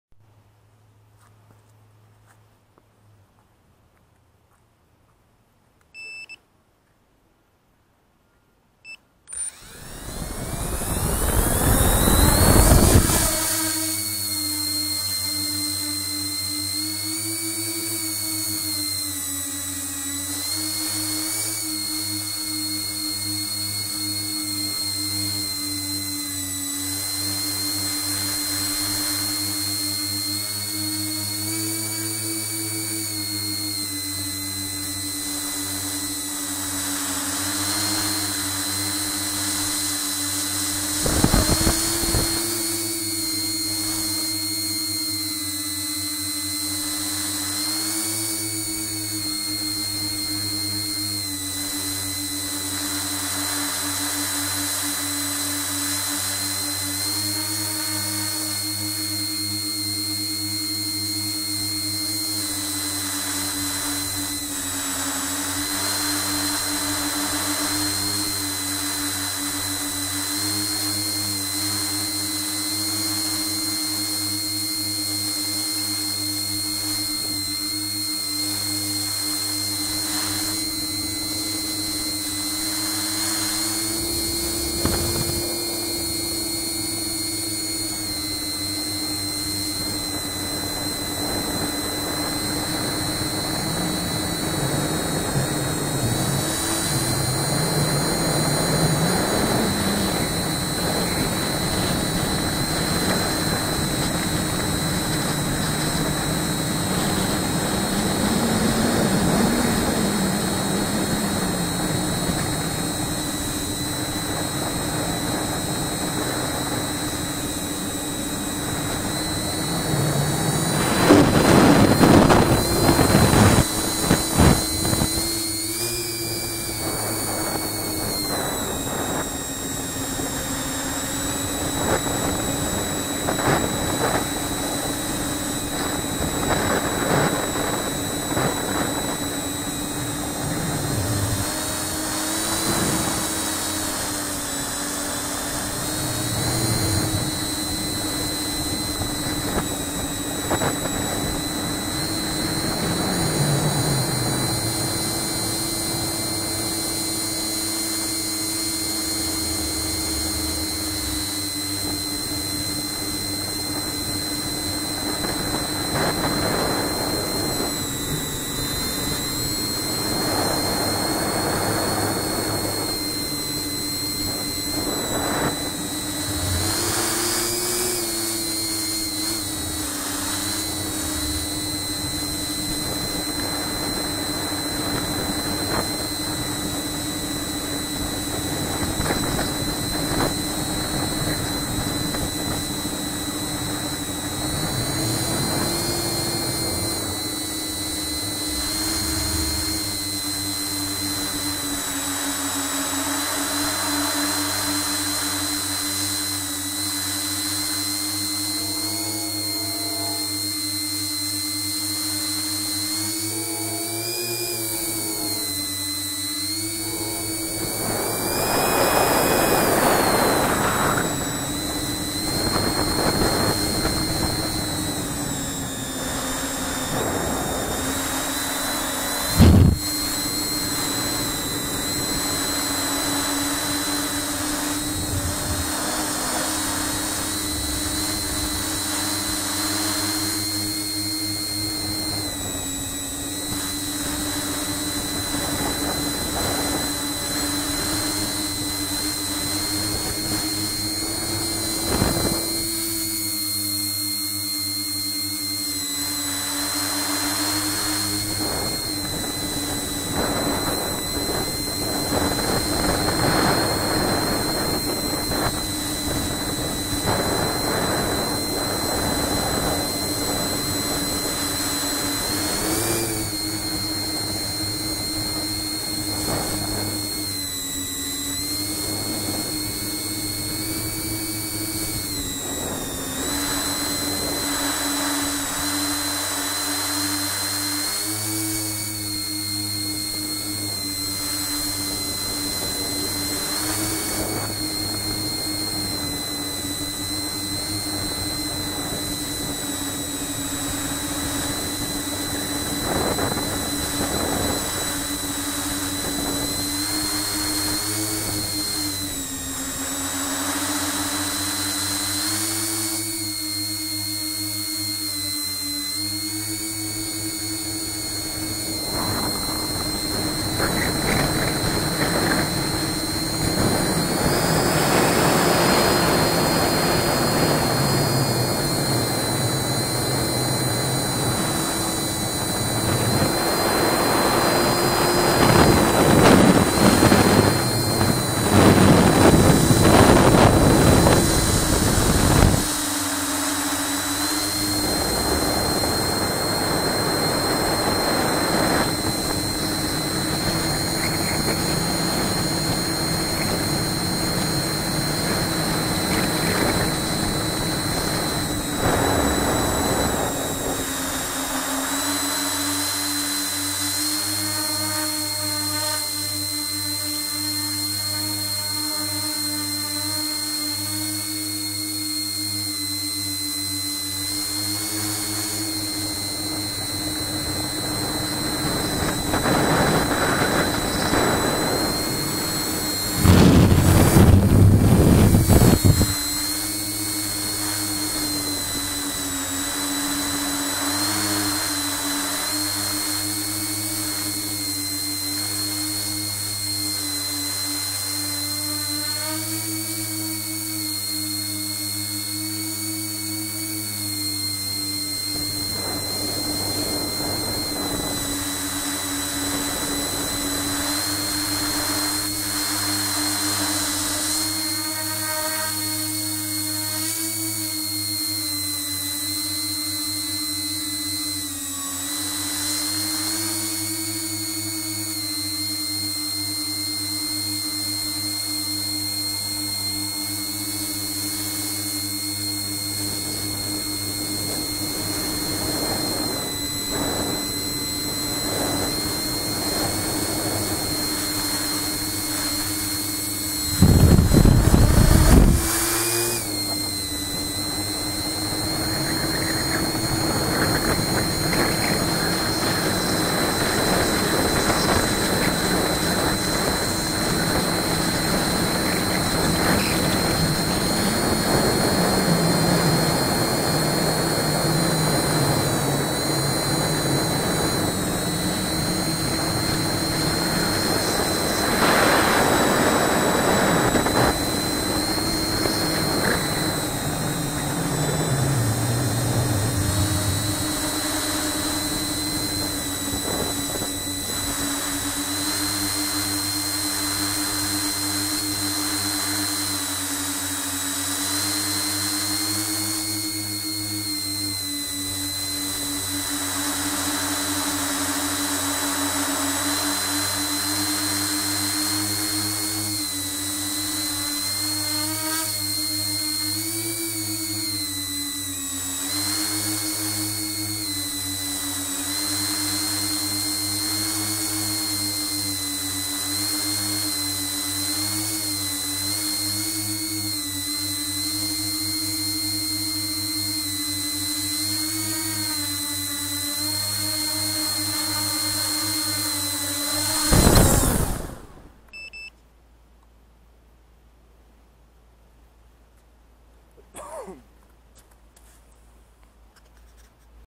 Tricopter 2 5 firefly 6s
First flight with spiderlite 18a esc's and graupner 9x5 tri blade props.